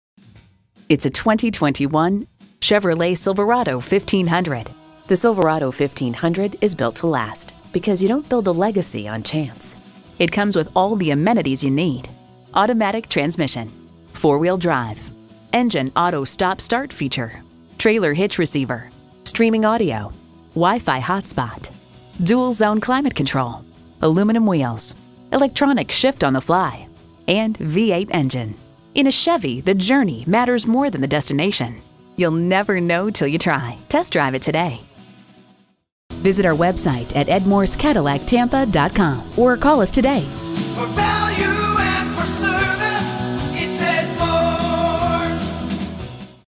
Used 2021 Chevrolet Silverado 1500 Tampa FL St. Petersburg, FL F133903A - SOLD
Year: 2021
Make: Chevrolet
Model: Silverado 1500
Trim: LT Trail Boss
Engine: 5.3Ll 8 Cylinder Fuel Injected
Transmission: 4WD Automatic
Color: Northsky Blue Metall
Interior: Black
Mileage: 32830
Stock #: F133903A
VIN: 1GCPYFED6MZ167793

Address:
101 East Fletcher Avenue